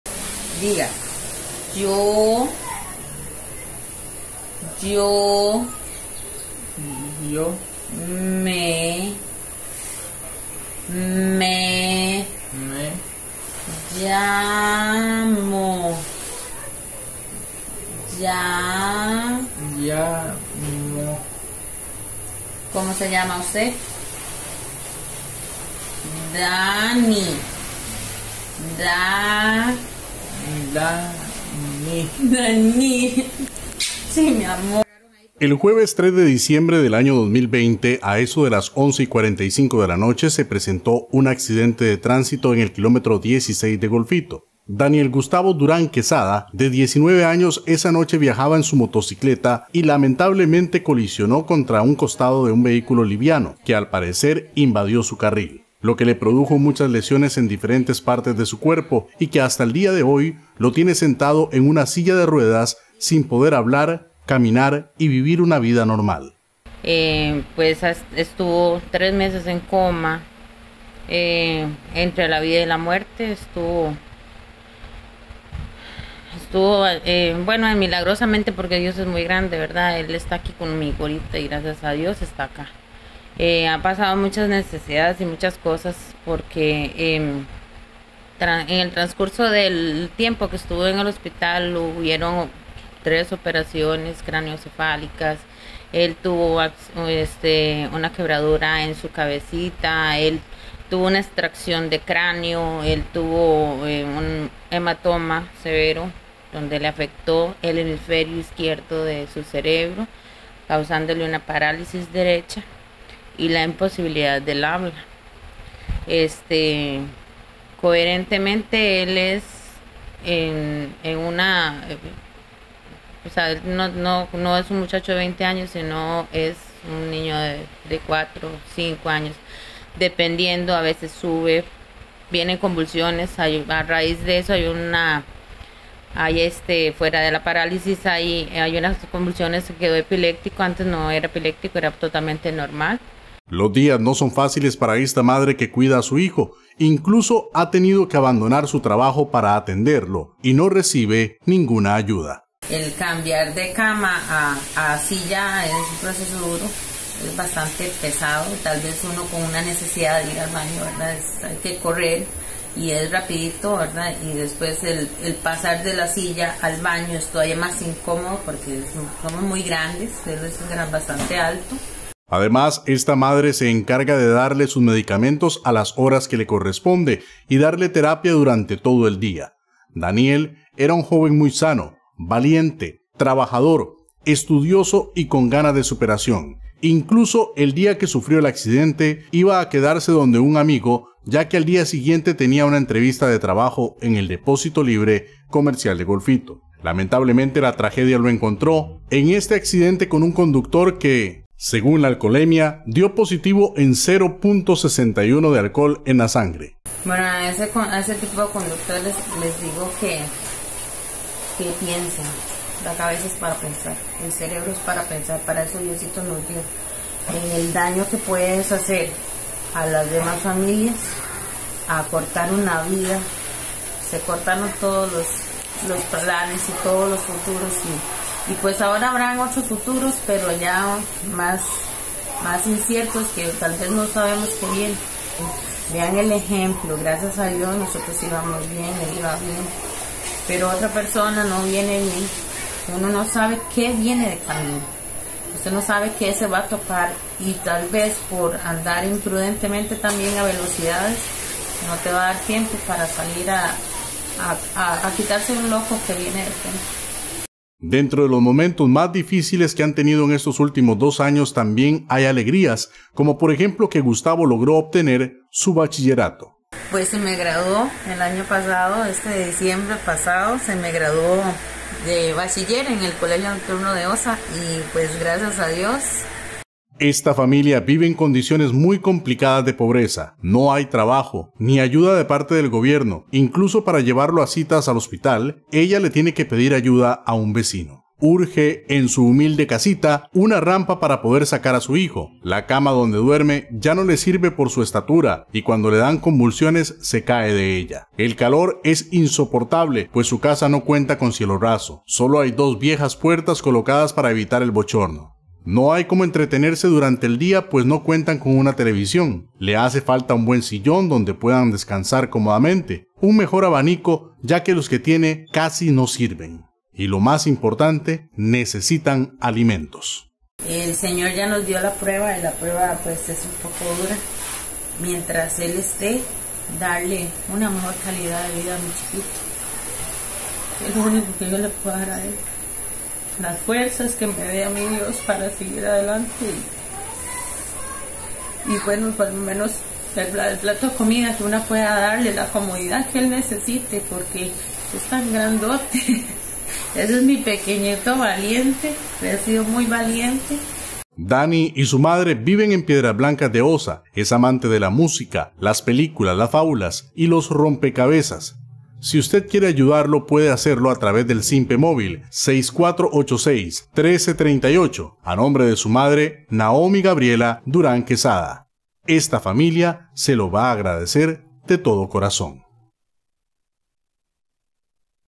JOVEN VECINO DE PIEDRAS BLANCAS SOBREVIVIO A ACCIDENTE EN SU MOTO
Un joven colisionó con su moto un automóvil que conducía un hombre que dio positivo a la alcoholemia. Desde entonces su vida y la de su madre no ha sido normal.

Requiere ayuda principalmente económica o en alimentos, si usted desea ayudarles puede llamar o enviar un sinpe al: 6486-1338.

Colaboró con esta información: Sidney Brown.